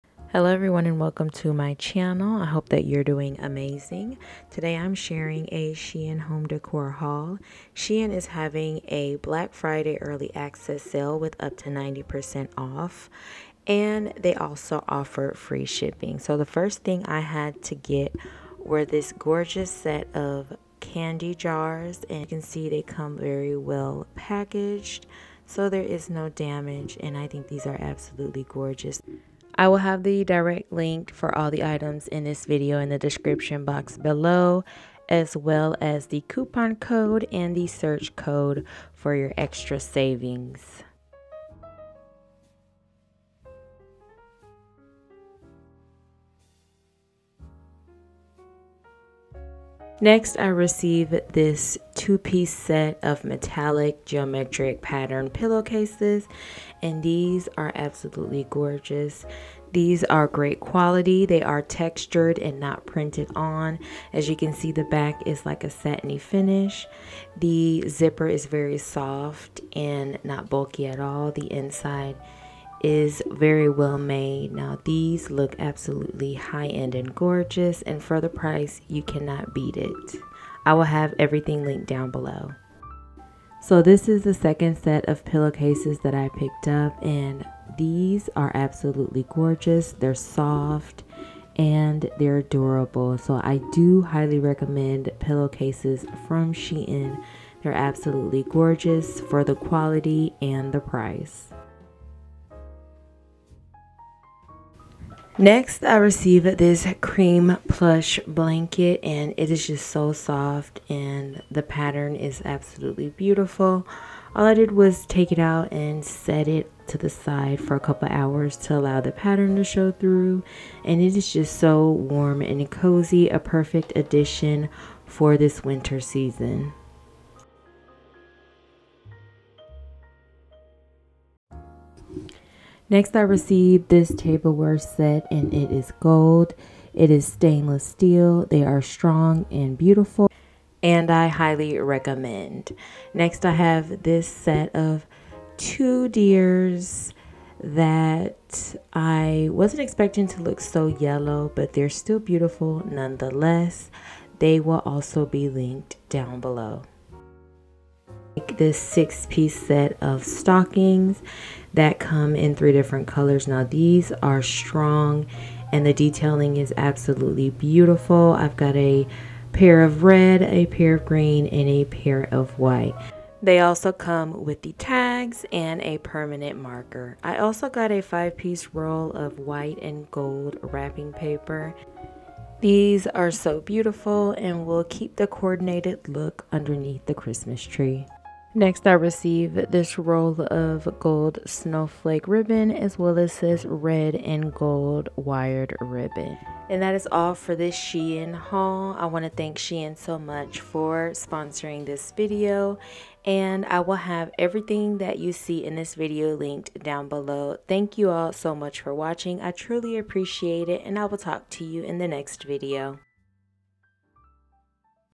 Elegant Shein Home Decor Haul | Shein Black Friday Early Access |DIY Glam Decor
Shein Black Friday Early Access Haul

Glitter Decorative Ribbons Red Gold Ribbon With Iron Wire Christmas Decoration Ribbon

TT: @shein_official @shein_us